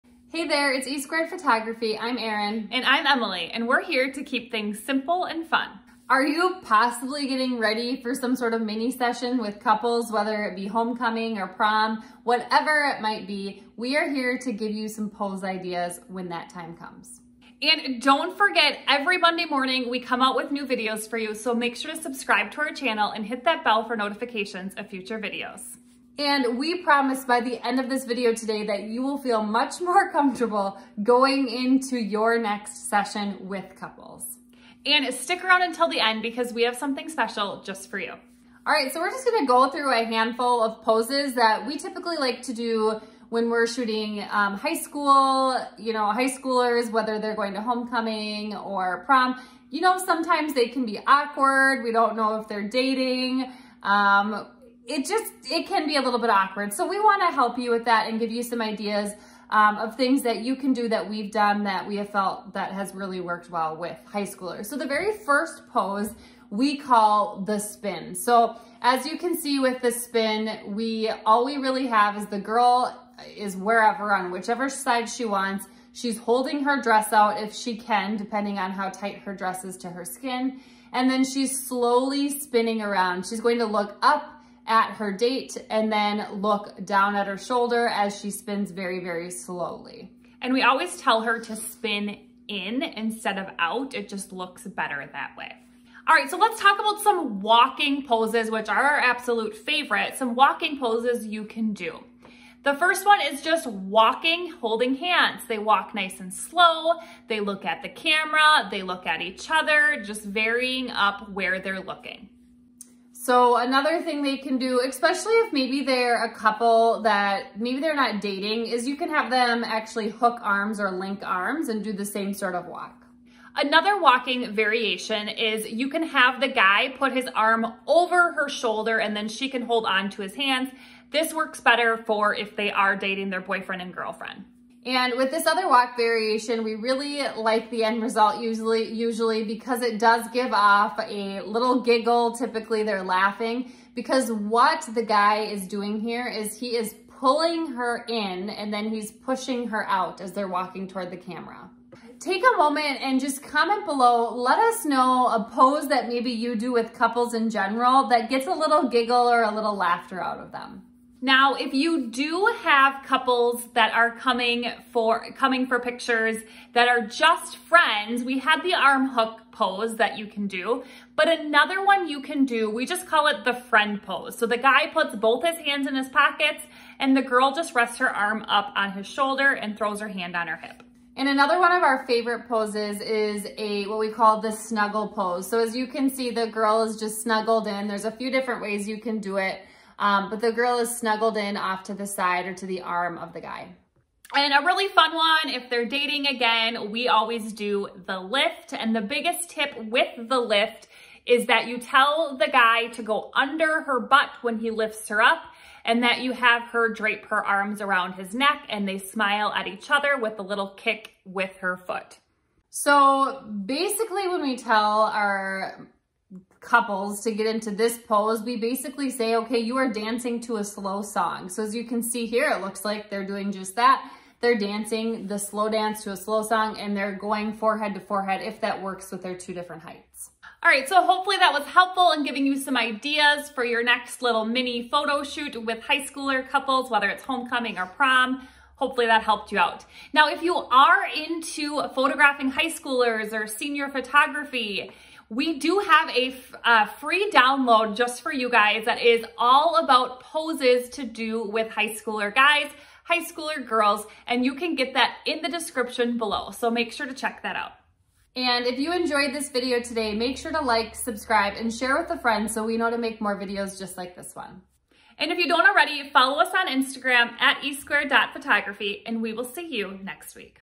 Couples Pose Ideas For Prom
COUPLES POSE IDEAS FOR PROM  // Are you organizing prom mini sessions or doing prom photos for a school? Maybe homecoming dance pictures? We are here to give you posing tips for couples. We are talking about how we pose couples for prom photos. Prom minis sessions are a great way to make some extra money and we are walking you through how to pose during these mini-sessions.

🎧 Watch this video next for more tips and tricks:  ▼